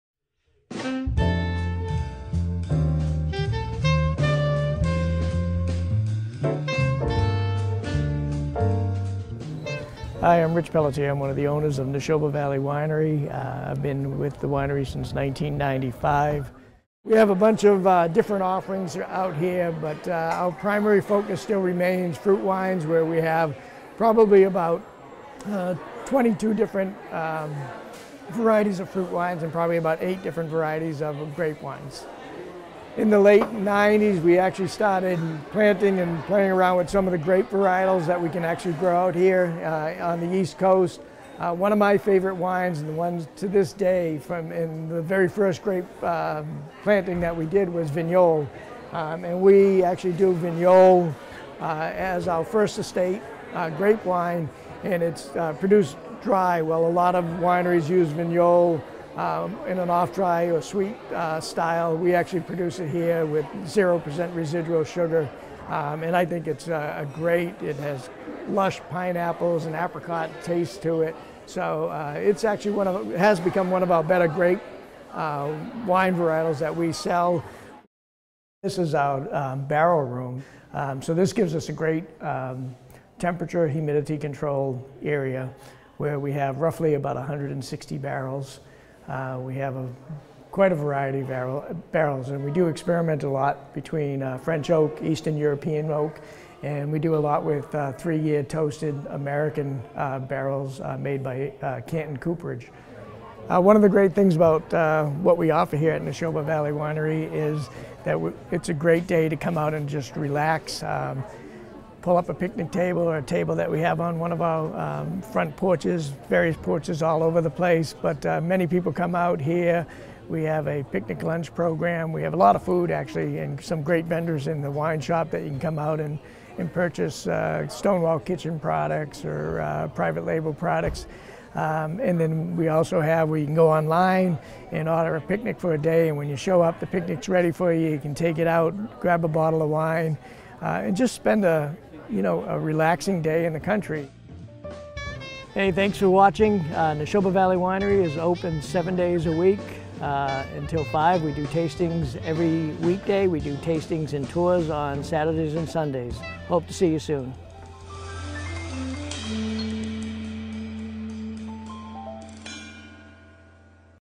Award-Winning Wines, Stunning Views at Nashoba Winery
Located in the heart of Massachusetts' apple country, Nashoba Valley Winery is one of the state's most recognized and awarded wineries.

Nashoba has a history dating back to 1980. Jack Partridge, the winery's founder, owned apple and pear trees in Somerville but had a dream to buy a New England farm and make fruit-based wines. He opened Nashoba Valley Winery out of a rented mill in Concord and in 1984 moved the winery to its current location - a sprawling 52 acre farm - in Bolton.

Partridge eventually decided to sell the winery and in conjunction with the commonwealth of Massachusetts and the Bolton Conservation Trust, the winery was sold to Richard and Barbara Pelletier in 1994.

In this video, Pelletier talks about the introduction and variety of grape wines that Nashoba produces including Vignole, Chardonnay and Merlot. Pelletier also discusses the importance of wood aging in Nashoba's fabulous barrel room and why Nashoba might just be the best place in the state for a family picnic, first date or simply a relaxing afternoon.

Nashoba Valley Winery is located at 100 Wattaquadoc Hill Rd. in Bolton, MA. The winery is open everyday from 10:00 AM to 5:00 PM. Guided tours of the wine-making facility are held every Saturday and Sunday beginning at 11:00 AM with additional tours conducted approximately every hour. The last tour of the day starts at 4:00 PM.

Editor's note: This video segment was produced by French Oak TV and is the ninth in a series profiling the Massachusetts Farm Winery & Growers Association and each of its member wineries.